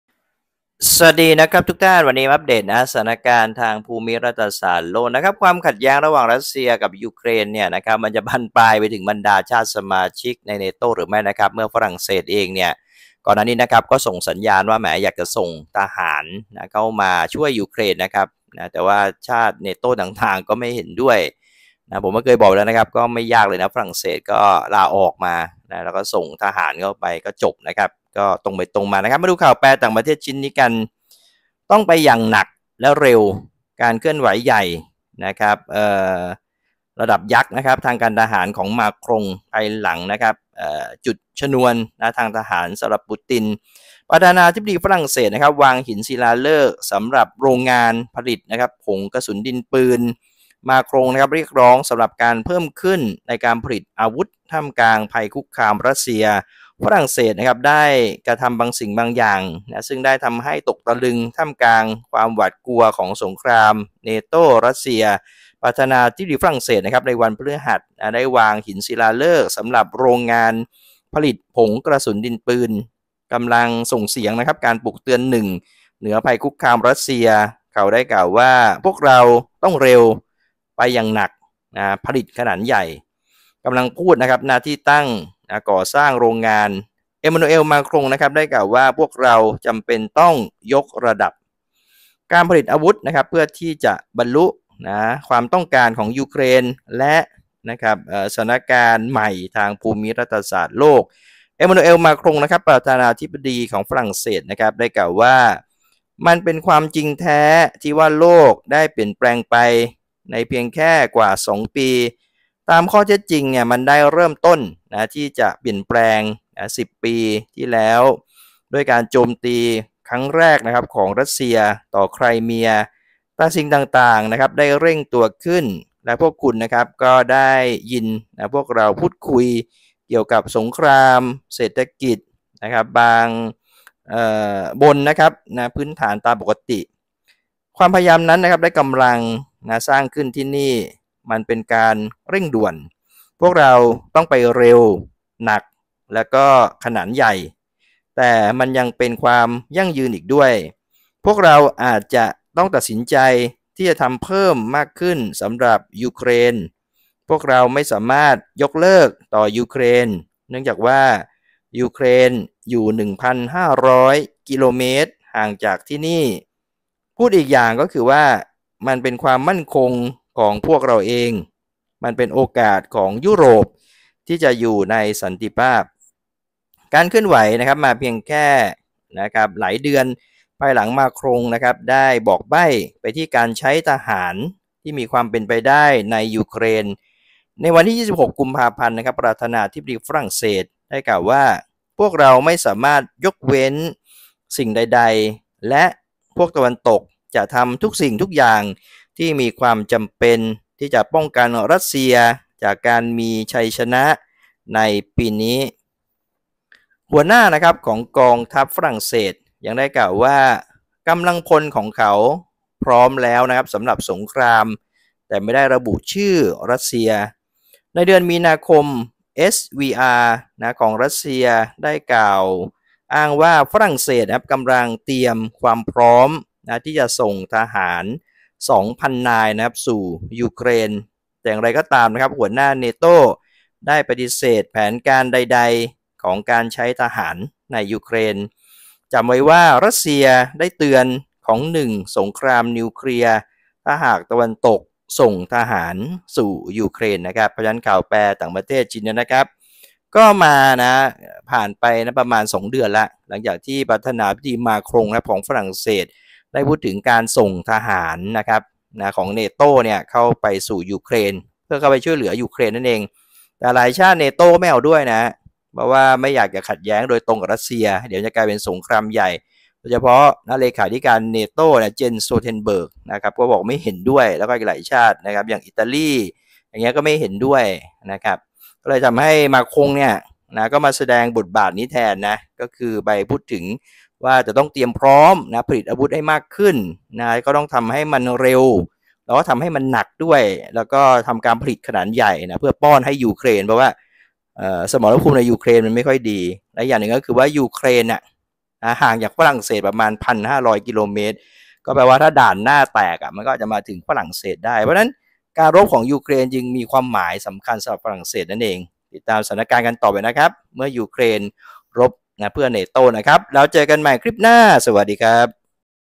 “ต้องไปอย่างหนัก&เร็ว” การเคลื่อนไหวใหญ่ยักษ์ทางการทหารของมาครง ภายหลังจุดชนวนทางทหารสำหรับปูติน?!
วิเคราะห์ เหตุการณ์ สถานการณ์ ภูมิรัฐศาสตร์โลก “ต้องไปอย่างหนัก&เร็ว” การเคลื่อนไหวใหญ่ยักษ์ทางการทหารของมาครง ภายหลังจุดชนวนทางทหารสำหรับปูติน?!